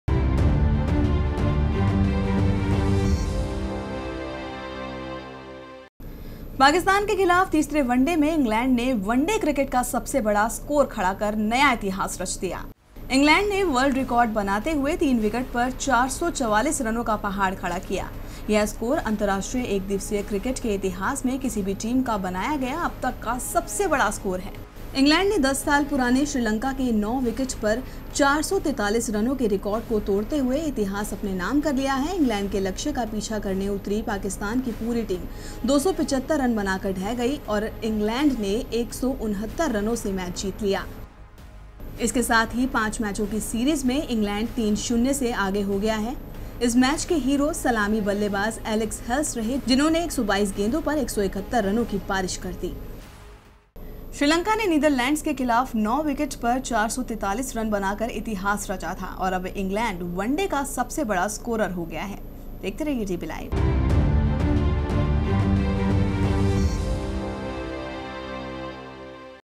DBLIVE | 31 August 2016 | ODI records get smashed by England at Trent Bridge
पाकिस्तान के खिलाफ तीसरे वनडे में इंग्लैंड ने वनडे क्रिकेट का सबसे बड़ा स्कोर खड़ा कर नया इतिहास रच दिया है।.. इंग्लैंड ने वर्ल्ड रिकॉर्ड बनाते हुए तीन विकेट पर 444 रनों का पहाड़ खड़ा किया। यह स्कोर अंतरराष्ट्रीय एकदिवसीय क्रिकेट के इतिहास में किसी भी टीम का बनाया गया अब तक का सबसे बड़ा स्कोर है। इंग्लैंड ने 10 साल पुराने श्रीलंका के नौ विकेट पर चार सौ तैतालिस रनों के रिकॉर्ड को तोड़ते हुए इतिहास अपने नाम कर लिया है...इंग्लैंड के लक्ष्य का पीछा करने उतरी पाकिस्तान की पूरी टीम 275 रन बनाकर ढह गई और इंग्लैंड ने 1 सौ उनहत्तर रनों से मैच जीत लिया। इसके साथ ही पांच मैचों की सीरीज में इंग्लैंड 3-0 से आगे हो गया है..इस मैच के हीरो सलामी बल्लेबाज़ एलेक्स हेल्स रहें...जिन्होंने 122 गेंदों पर 171 रनों की बारिश कर दी।